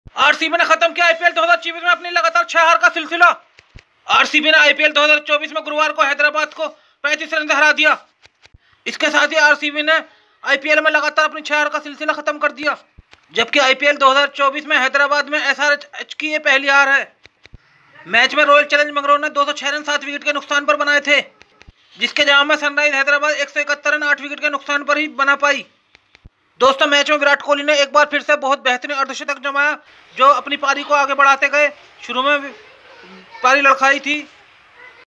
रॉयल चैलेंजर्स बैंगलोर ने खत्म किया अपनी लगातार हार का सिलसिला
विराट कोहली की शानदार अर्धशतक और जयदेव उनादकट की घातक गेंदबाजी की बदौलत रॉयल चैलेंजर बेंगलुरु ने सनराइज हैदराबाद को 35 रन से हरा दिया और अपनी लगातार हार का सिलसिला खत्म कर दिया viratkohli cricket crickethighlights ipl cricketnews @tttgamin